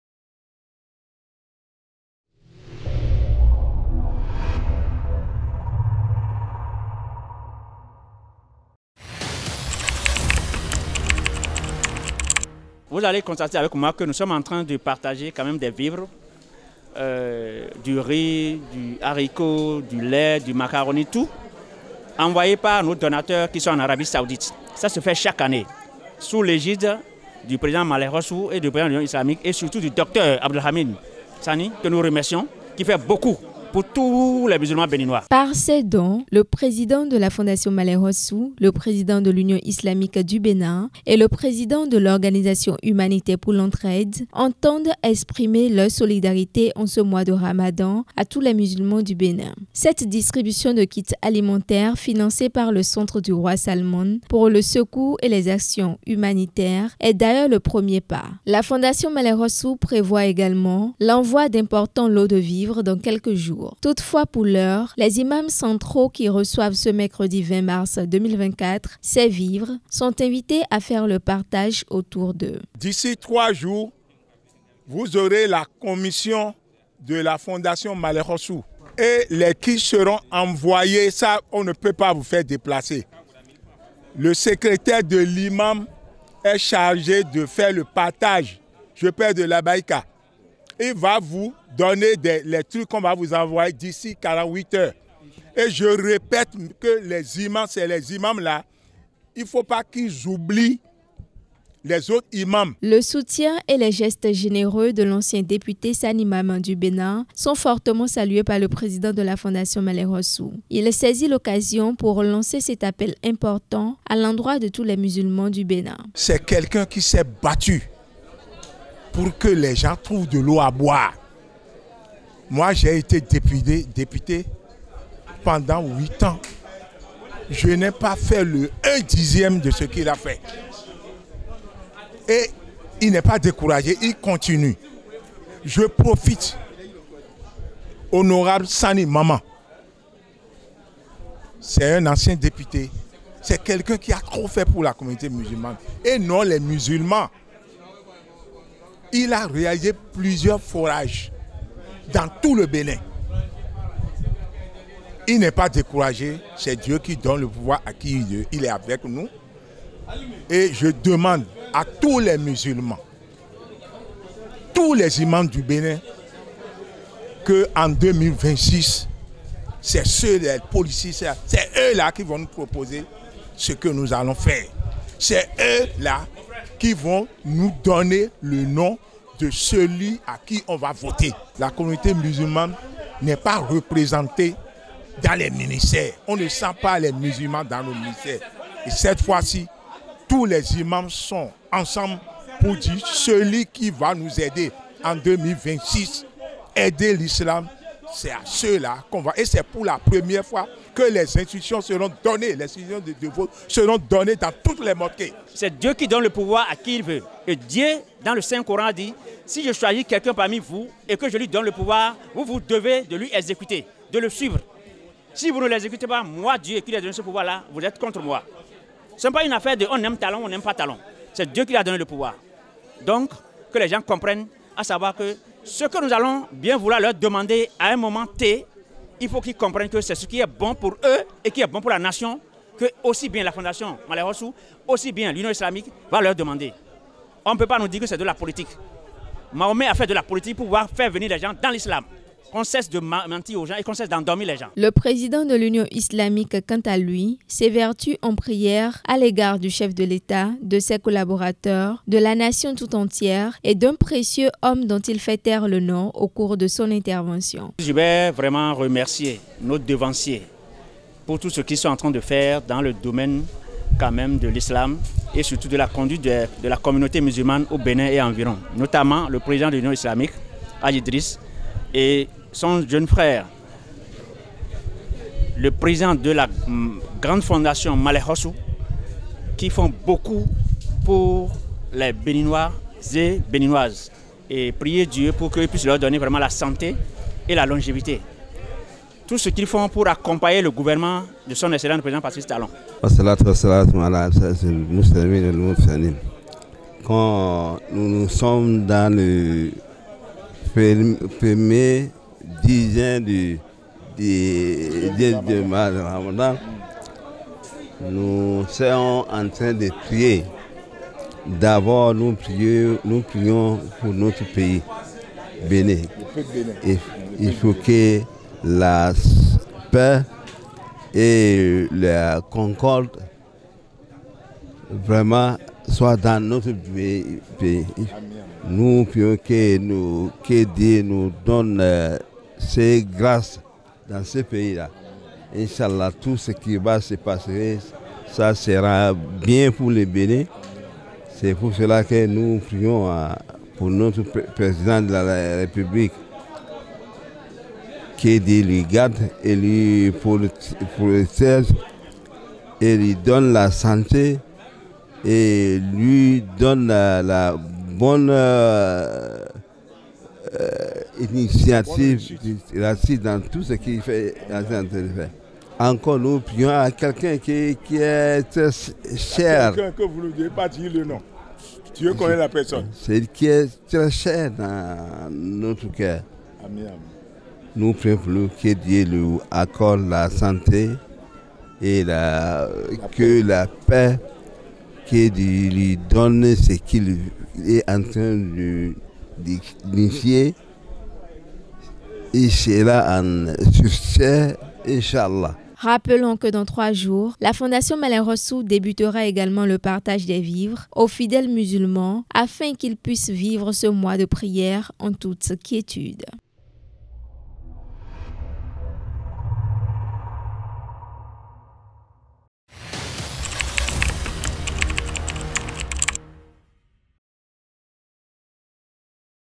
Distribution de kits alimentaires du centre du roi Salmane
Distribution de kits alimentaires financé par le centre du roi Salmane pour le secours et les actions humanitaires aux fidèles musulmans ce mercredi 20 mars 2024 à BOHICON. Le président de la fondation Malehossou et le président de l’union islamique du bénin ont remis d’importants lots de vivres aux musulmans.